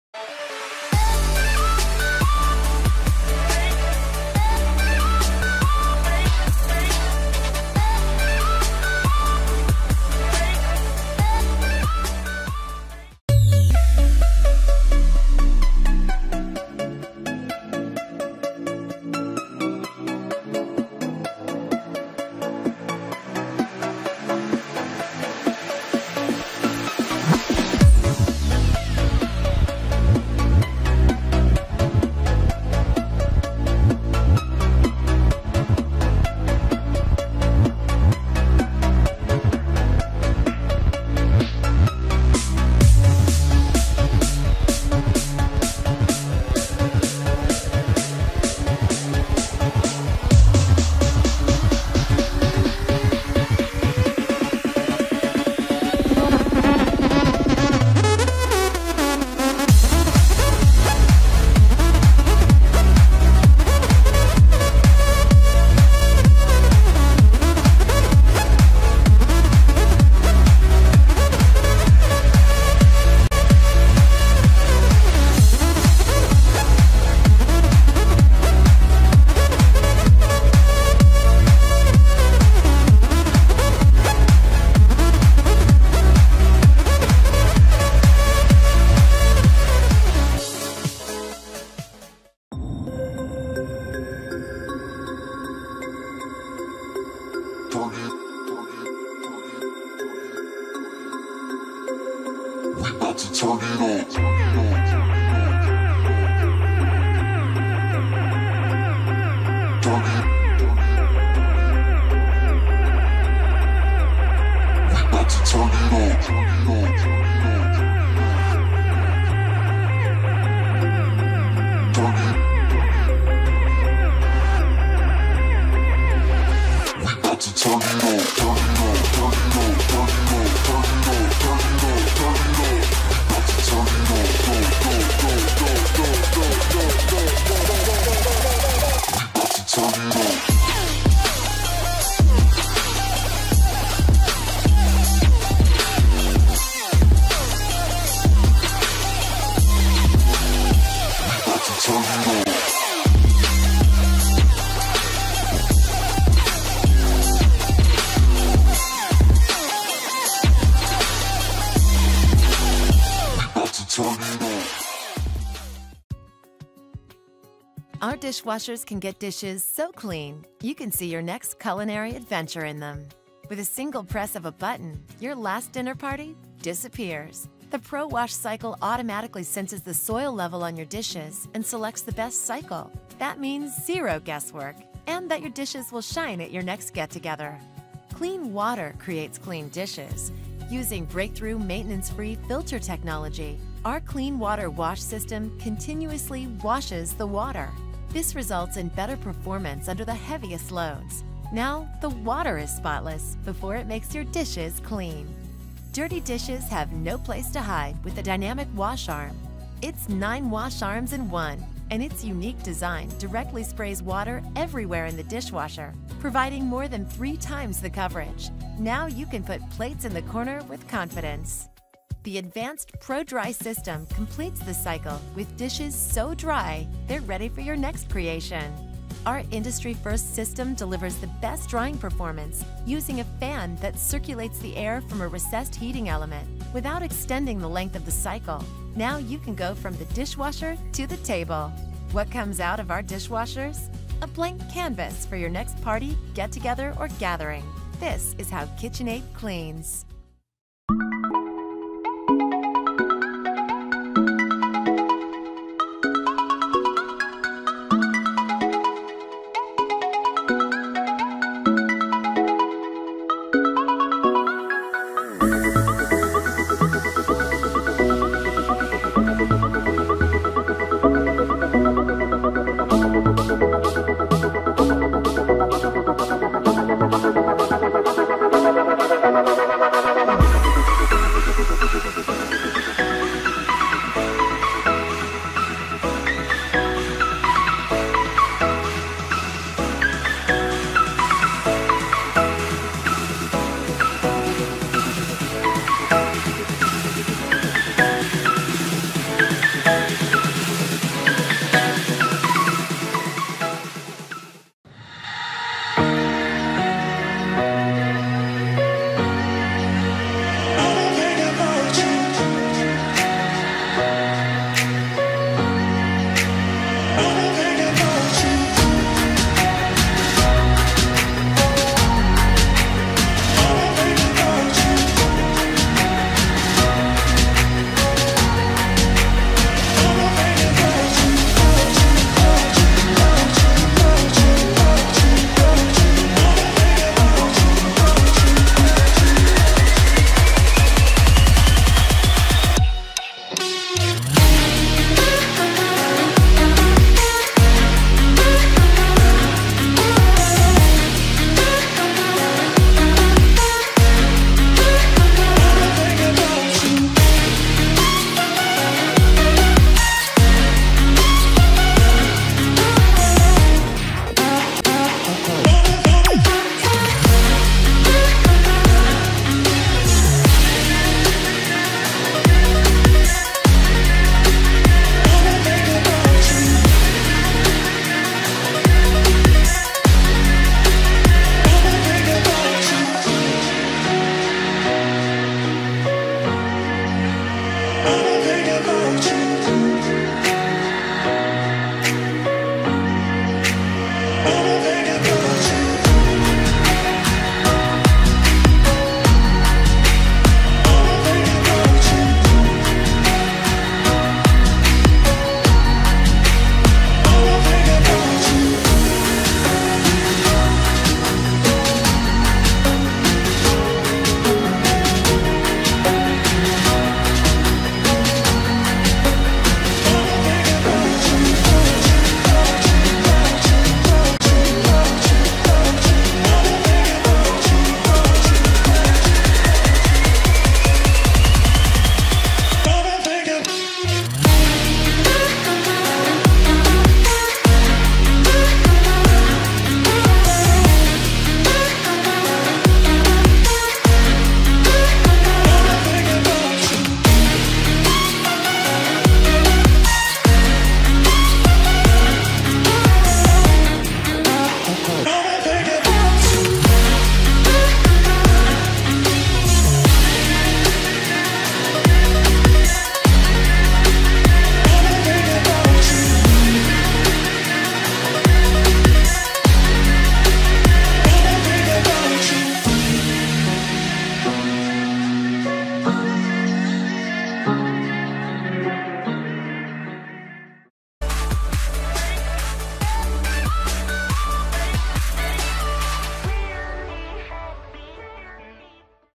Top 5 Best Dishwasher to Buy in 2021
In this video we shared top 5 best dishwasher.

5 ► Miele Futura Classic Series G4205SS Dishwasher
Amazon US
4 ► Bosch 800 Series Dishwashers
Amazon US
3 ► kitchenaid Dishwasher
Amazon US
2 ► Maytag Dishwasher
Amazon US
1 ► LG Quadwash Dishwasher
Amazon US